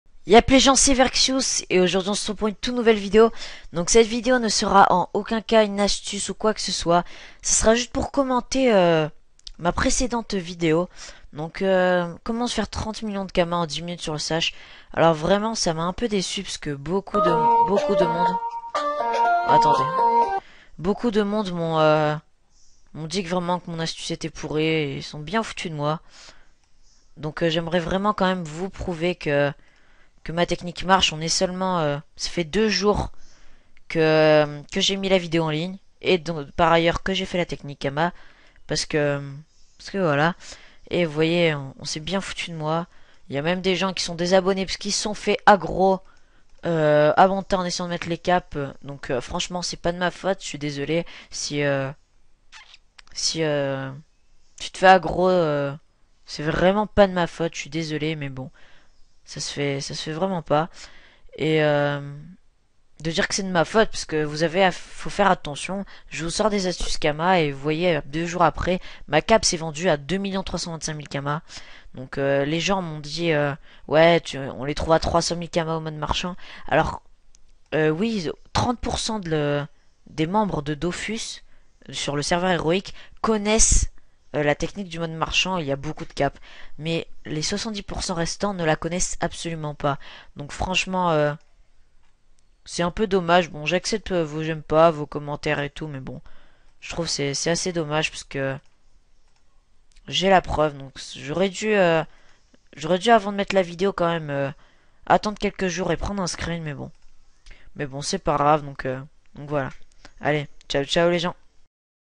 Preuve que mon astuce kamas 12# MARCHE !!!
### Voila voila beaucoup d'entre vous se sont foutus de moi critiquant ma dernière technique kamas, par cette vidéo je vous prouve qu'elle marche !  ###